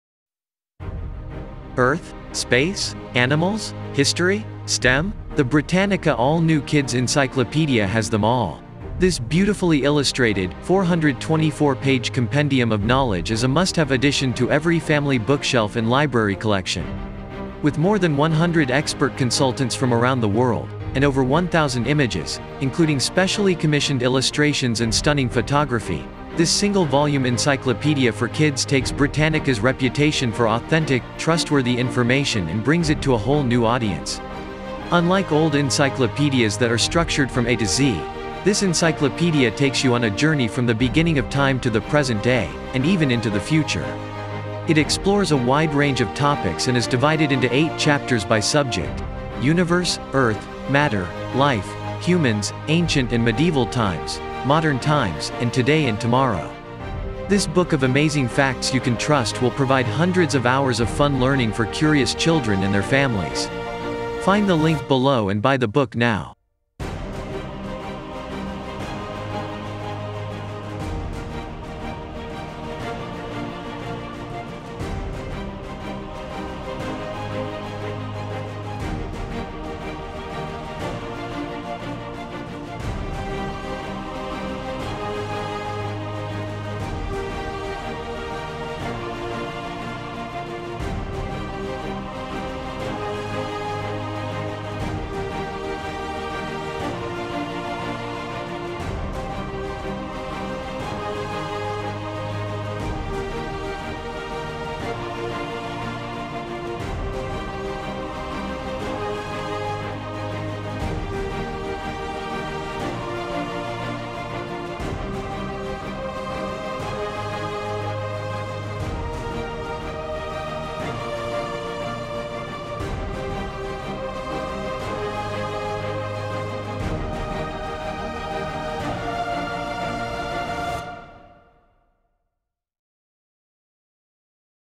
Britannica All New Children's Encyclopedia
Britannica All New Children's Encyclopedia: What We Know and What We Don't

Earth? Space? Animals? History? STEM? The Britannica All New Children's Encyclopedia has them all.

This beautifully illustrated, 424-page compendium of knowledge is a must-have addition to every family bookshelf and library collection!

With more than 100 expert consultants from around the world, and over 1000 images, including specially commissioned illustrations and stunning photography, this single-volume encyclopedia for kids takes Britannica’s reputation for authentic, trustworthy information and brings it to a whole new audience.

Unlike old encyclopedias that are structured from A to Z, this encyclopedia takes you on a journey from the beginning of time to the present day – and even into the future! It explores a wide range of topics and is divided into eight chapters by subject: Universe, Earth, Matter, Life, Humans, Ancient & Medieval Times, Modern Times, and Today & Tomorrow.

This book of amazing facts you can trust will provide hundreds of hours of fun learning for curious children and their families.